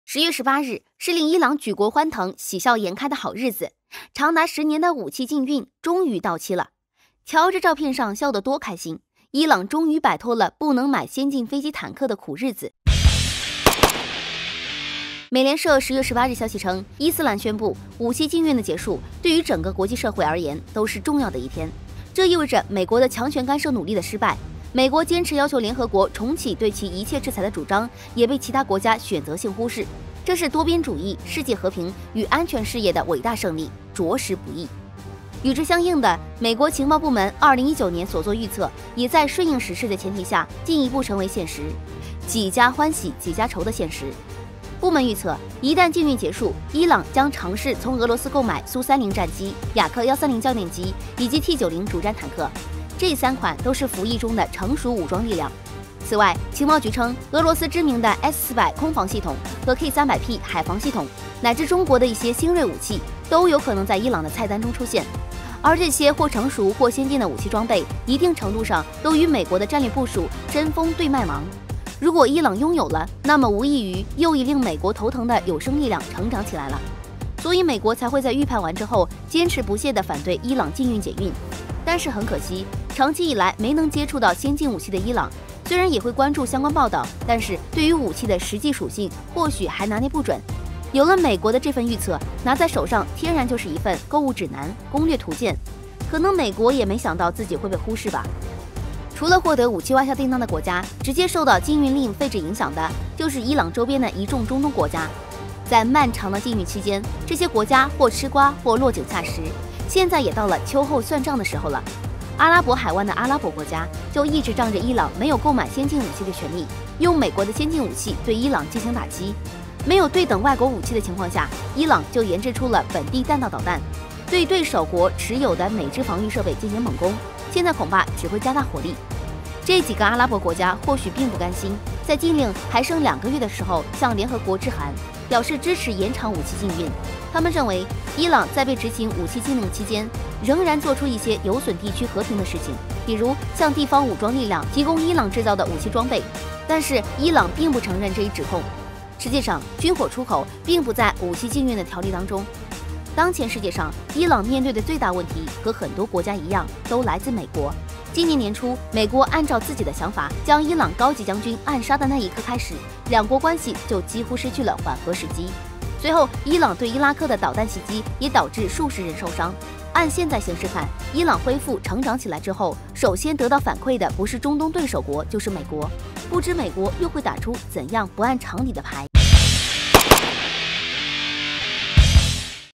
伊朗舉國歡騰，武器禁運終於到期，中國新銳武器或出現在“菜單”【壹號哨所】
10月18日，是令伊朗舉國歡騰、喜笑顏開的好日子——長達十年的武器禁運終於到期了——瞧這照片上笑得多開心，伊朗終於擺脫了不能買先進飛機坦克的苦日子。 我們的影片會遵循公平、尊重的原則，確保不會包含任何仇恨言論的元素。